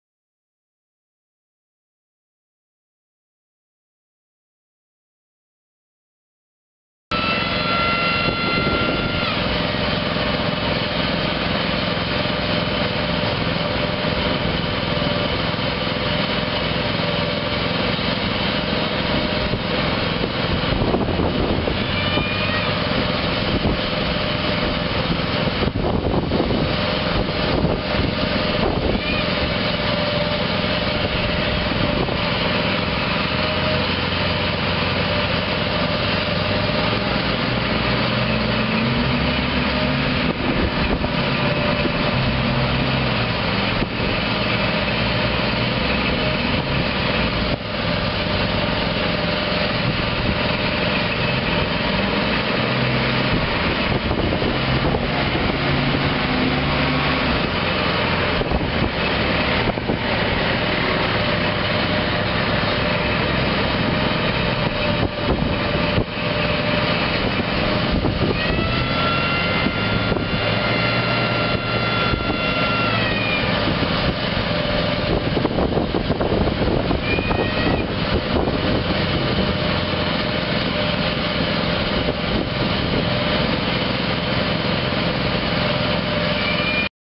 1999 Snorkel TB60 boom lift for sale | sold at auction March 27, 2014
1999 Snorkel TB60 boom lift
3,129 hours on meter
Model TB-A60RF0
Ford LRG425 four cylinder dual fuel engine
Non-operational engine
60&#039; maximum platform height
92" x 30" rotating platform
15-19.5 NHS foam filled tires
21,040 lbs. base operating weight
Manuals
Serial 991524
Unit# 1524